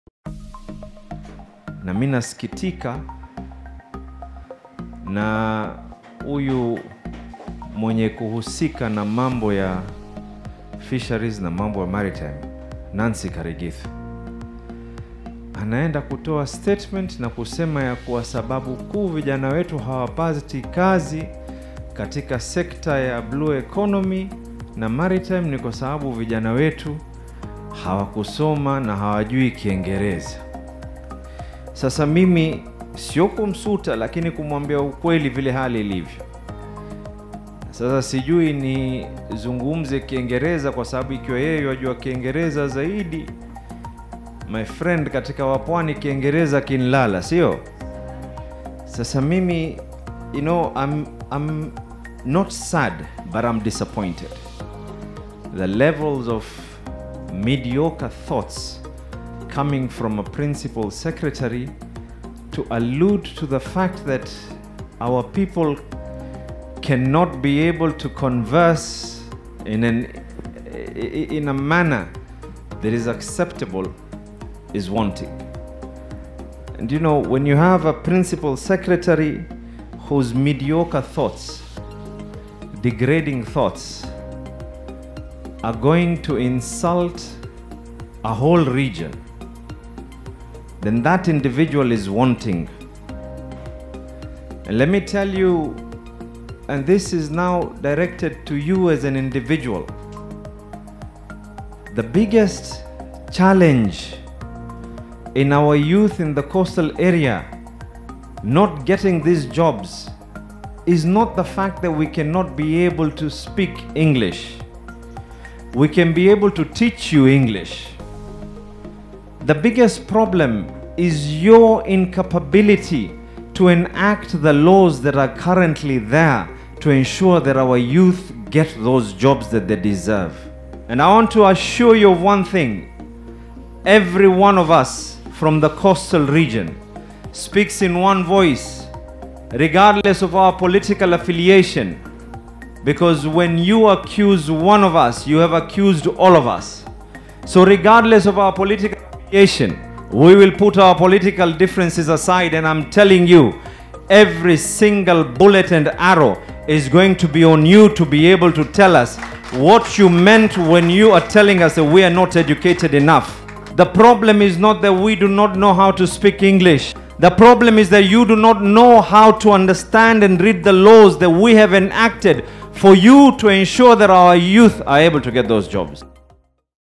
Nancy Karigithu on the spot for saying lack of English skills bars coastal youth from maritime jobs
Mvita MP Abdulswamad Shariff slams PS Nancy Karigithu regarding remarks she made saying coastal youths failed to secure maritime jobs due to inability to speak fluent English.